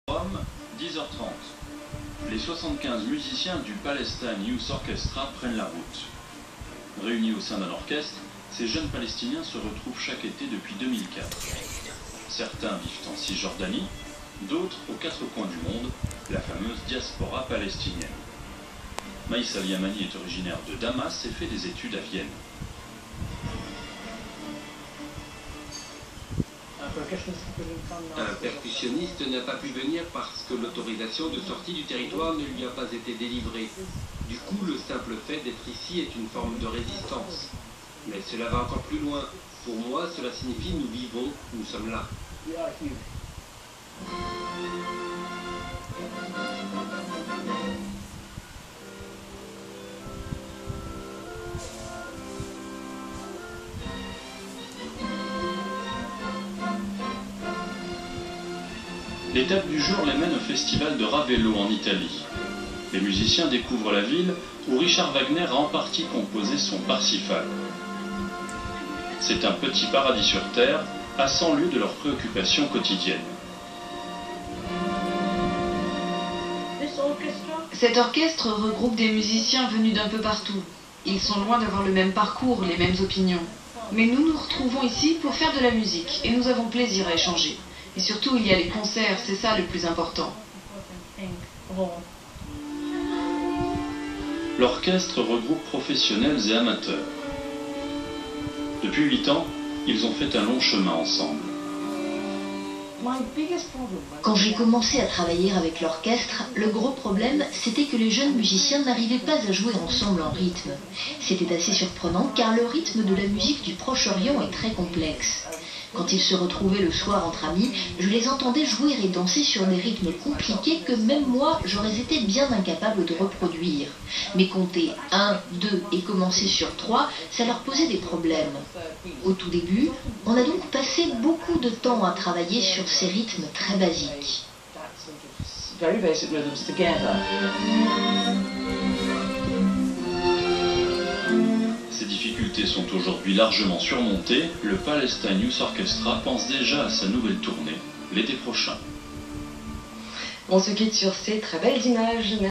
PYO ravello festival.mp4
TV program (arte channel) on Palestinian youth orchestra concert on August 2 in Ravello